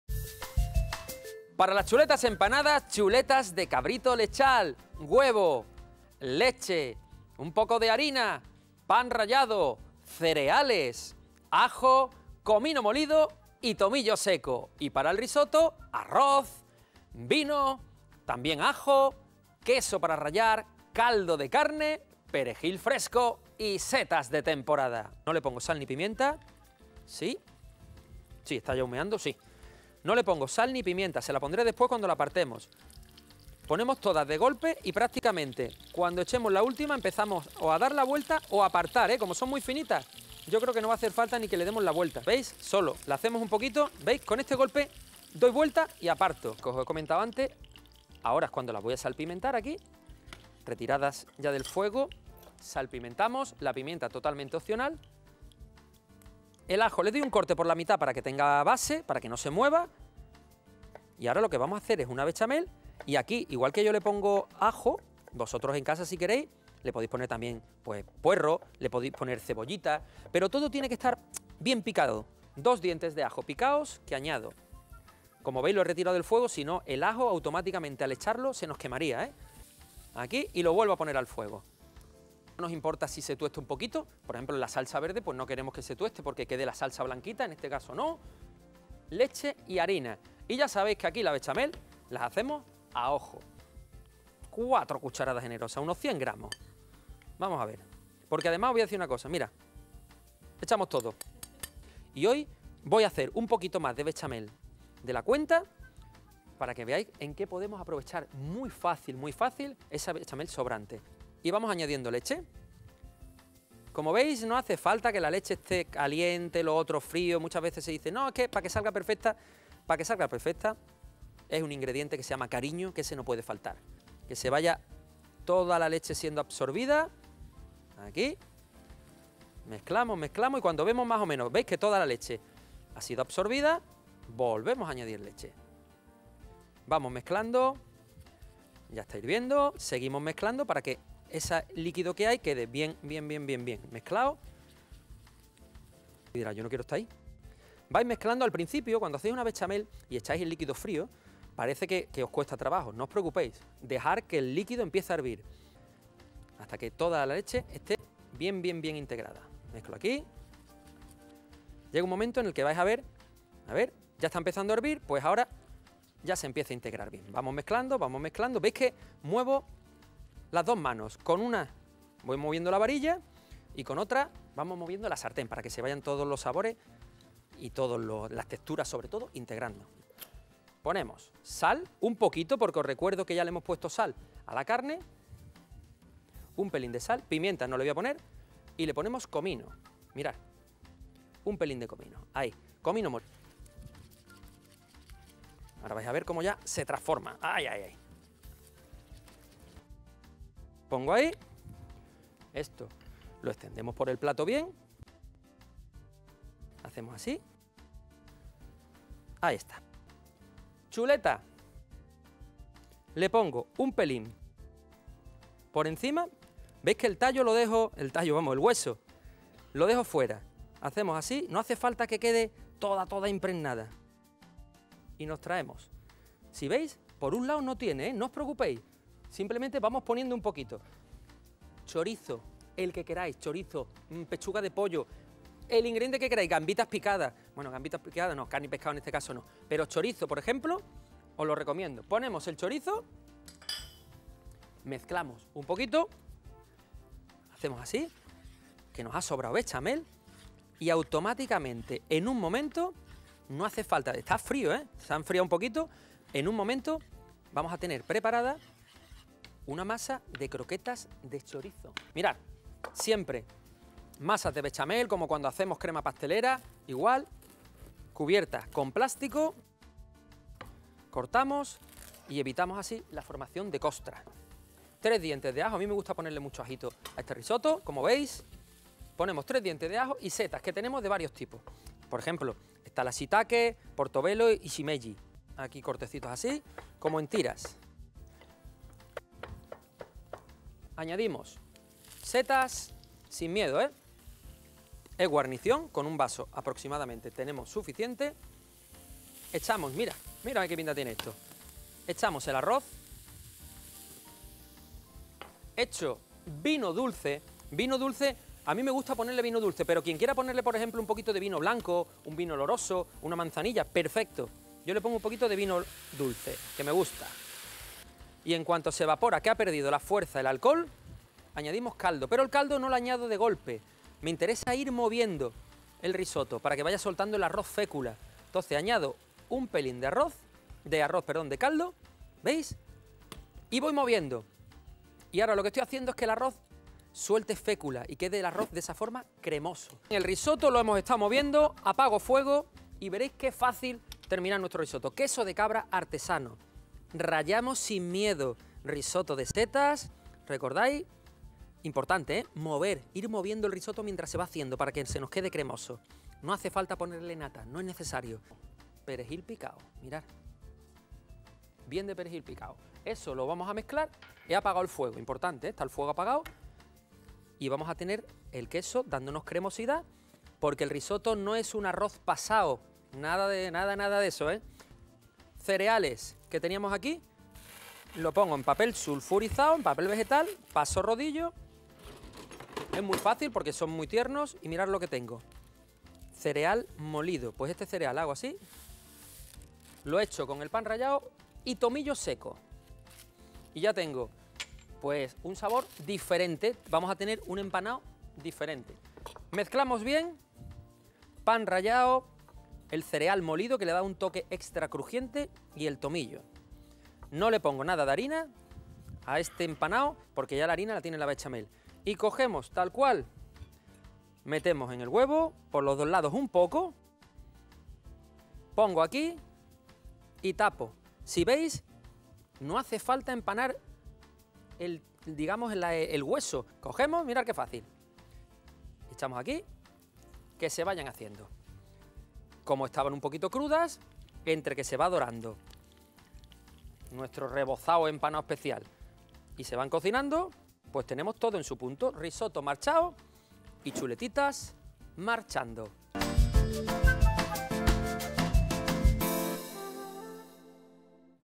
Chuletas empanadas y risotto
INGREDIENTES:
Para las chuletas Villaroy:
1 costillar de cabrito lechal
2 dientes de ajo
100 g. de harina
2 vasos de leche
3 huevos
1 vaso de cereales de maíz
½ vaso de pan rallado
comino molido y tomillo seco

Para el risotto de setas y queso de cabra:
1 cuña de queso de cabra para rallar
3 dientes de ajo
200 g. de setas de temporada
1 vaso de arroz
1 copita de vino dulce
2 vasos de caldo de carne
perejil picado
aceite de oliva virgen extra, sal y pimienta.

ELABORACIÓN:
Cocinar brevemente a fuego fuerte, en una sartén con aceite de oliva virgen extra, las chuletillas; debiendo quedar poco hechas. Apartar, salpimentar y reservar.
En la misma sartén, hacemos una bechamel:
Para ello, rehogar 2 dientes de ajos picados. Añadir 100 g. de harina y cocinar un par de minutos sin parar de mover. Verter entonces poco a poco la leche, una pizca de comino molido y salpimentar. Dejar que hierva, mantener unos minutos al fuego y retirar del calor.
Coger las chuletillas por la parte final del hueso e introducirlas dentro de la bechamel hasta que la carne esté bien cubierta. Colocar sobre una fuente engrasada con aceite de oliva y reservar al frío.
Picar el resto de los dientes de ajo y comenzar a rehogarlos en aceite de oliva virgen extra con las setas troceadas. Agregar 1 vaso de arroz y sofreír brevemente.
Incorporar el vino, dejar que se evapore el alcohol y comenzar a agregar caldo de carne, poco a poco, moviendo de forma constante y dejando que el arroz vaya absorbiendo el caldo.
Cuando el arroz esté en su punto, retiramos del fuego, probamos de sal, rallamos abundante queso de cabra por encima y espolvoreamos con perejil picado. Mezclar suavemente y dejar reposar 2-3 minutos.
Una vez que están bien frías las chuletillas, las empanamos; pasándolas por huevo batido y una mezcla de pan rallado, cereales molidos y tomillo seco.
Freímos en aceite de oliva y apartamos sobre papel de cocina para escurrir.